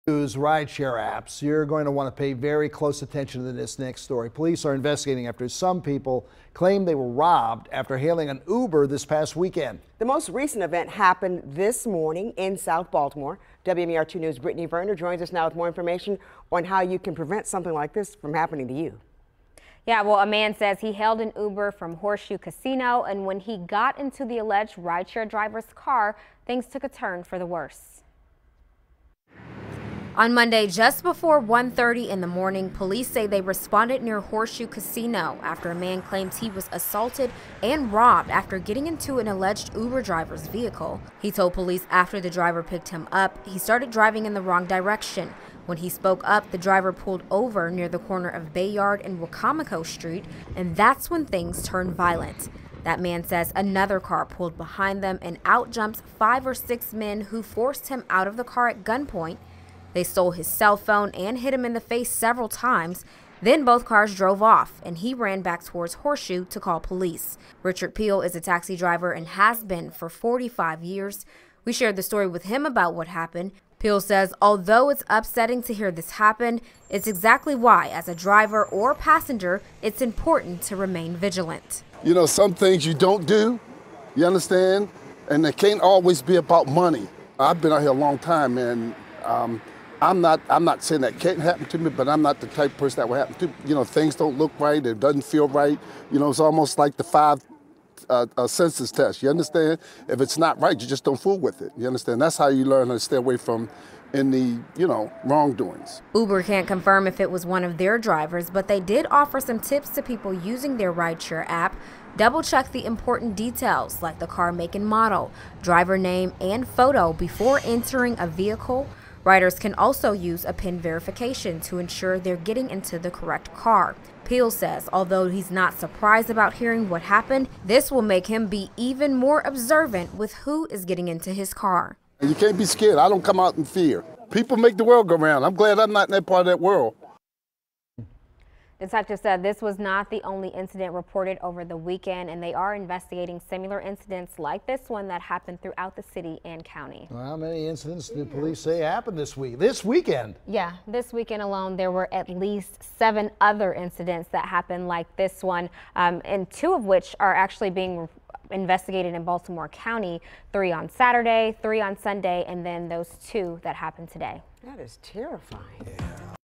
Baltimore Uber riders targeted in recent string of violence
Uber riders have been the recent target of violence in Baltimore City.

Over the weekend police received several reports of people posing as Uber drivers assaulting and robbing customers.

The latest incident happened Sunday, just before 1:30am outside Horseshoe Casino.

Police say a man picked up what he thought was an Uber from the valet area.

After taking the victim in the wrong direction, the so called Uber driver reportedly pulled over in the 1600 block of Bayard Street.

That's when the victim claims a second car pulled up from behind with between 5-6 armed assailants.